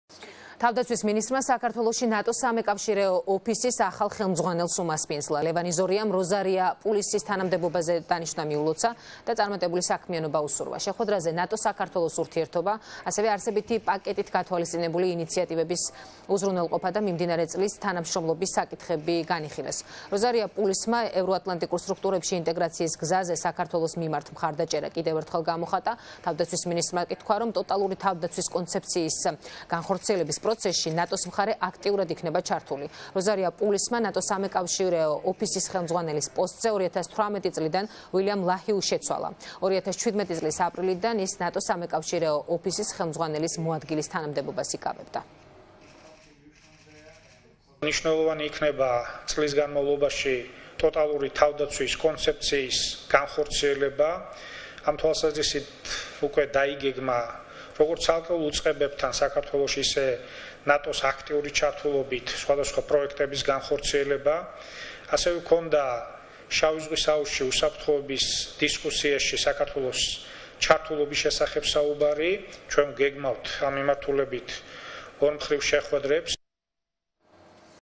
მაესტრო 15:25
თავდაცვის მინისტრის შეხვედრა ნატოს სამეკავშირეო ოფისის ხელმძღვანელთან